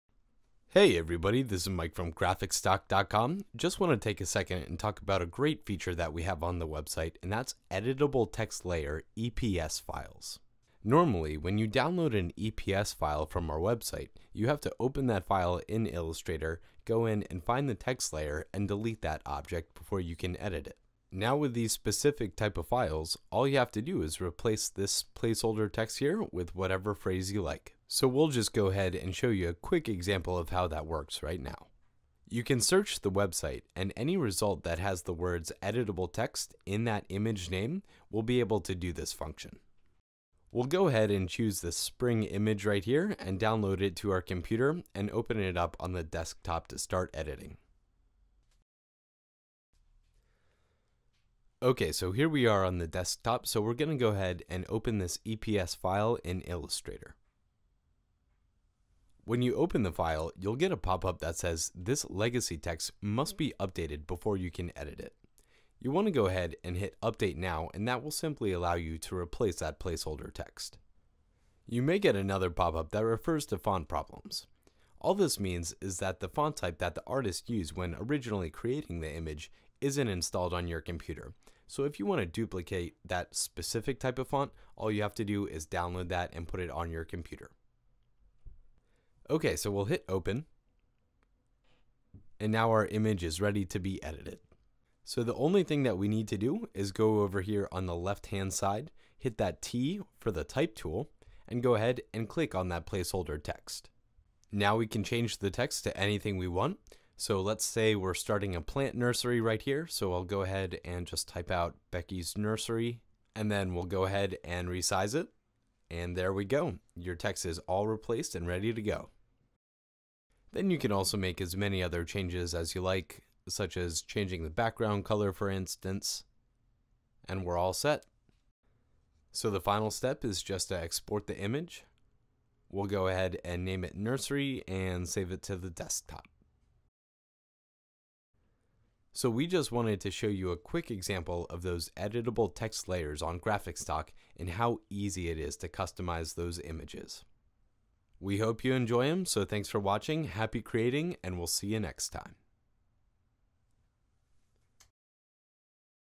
How to Edit Text in Vector Files from GraphicStock
Open the AI or EPS file in Adobe Illustrator. Select the Type Tool from the toolbar on the left, then click the text that you want to edit. Replace the current text with your desired text. To format the font, select the text and change the color, font  family, size, etc. using the property bar located under the menu bar. You can find more options if you open the character and paragraph panels. To pull these up, go to "Window" - "Type" - "Character/Paragraph."

📹 Create better video, faster than ever with unlimited stock media downloads ➡️